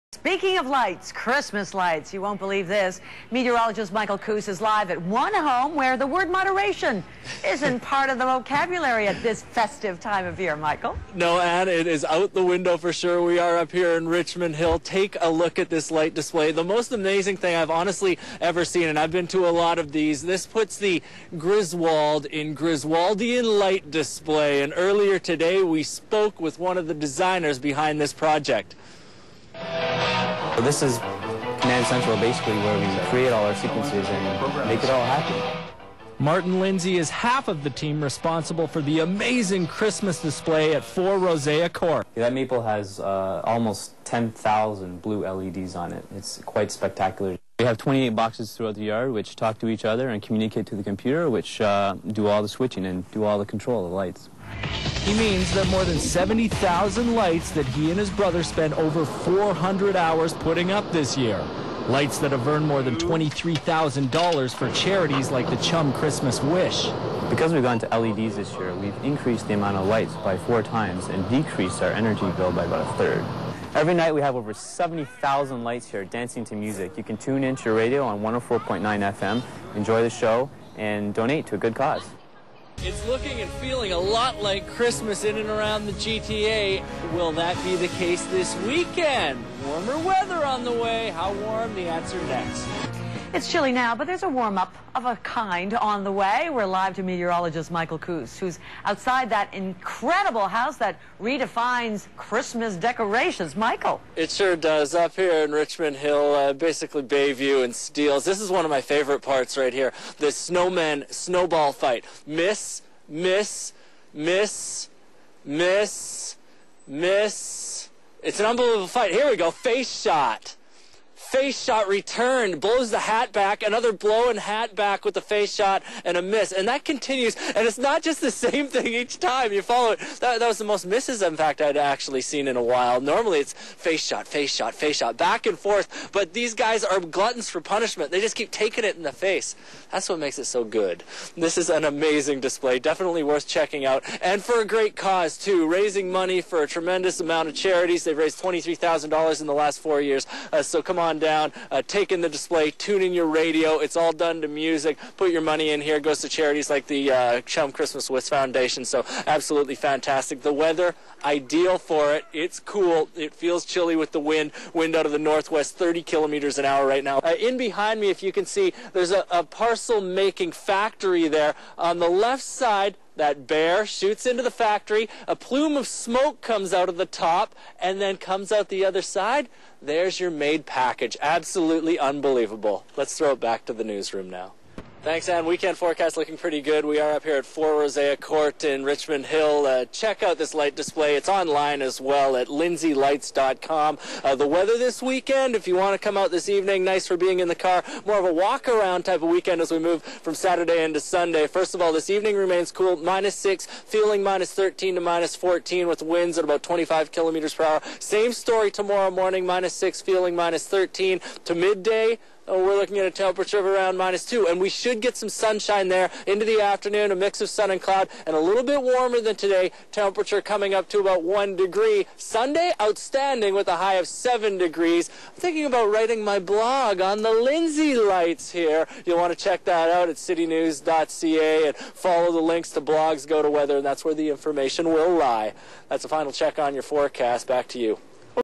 Lindsay Lights
2006 CityTV coverage of Lindsay lights.